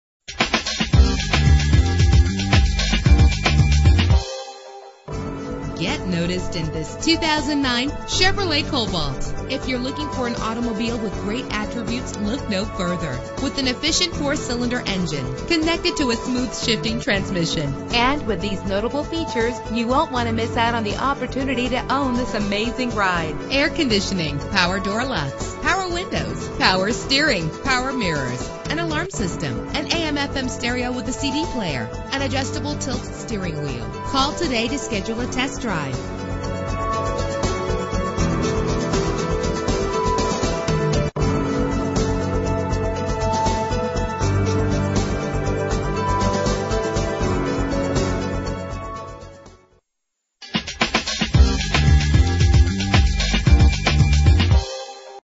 2009 Chevrolet Cobalt - Vestal NY
Year: 2009
Make: Chevrolet
Model: Cobalt
Trim: LT
Engine: 2.2 liter inline 4 cylinder 16 valve
Transmission:
Color: Blue
Mileage: 40781
Address:
3721 Old Vestal Rd